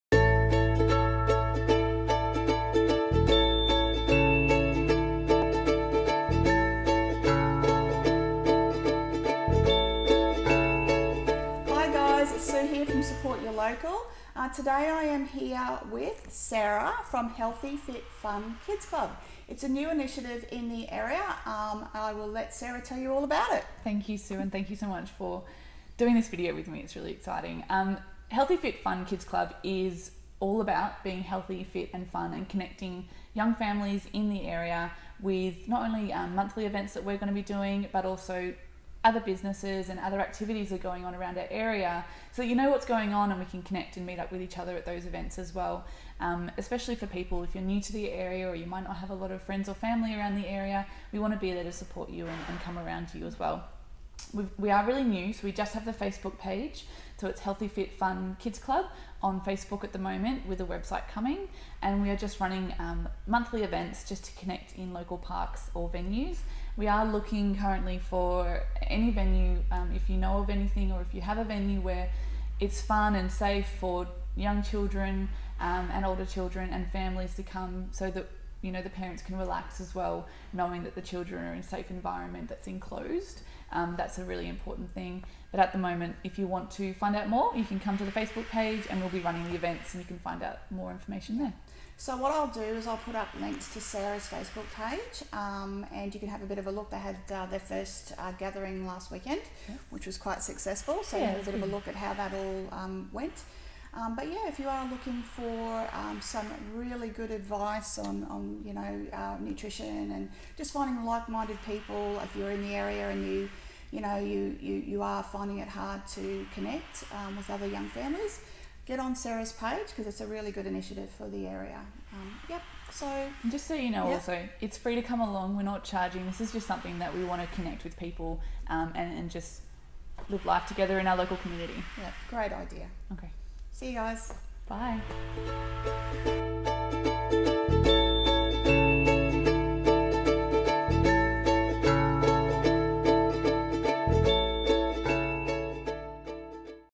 Support Your Locals - Healthy Fit Fun Kids Club
Today we met with Sarah from Healthy Fit Fun Kids Club.

Kids Club is all about encouraging through connecting families and kids to inspire healthy living and having fun. We want to connect and create community in the Ormeau Area.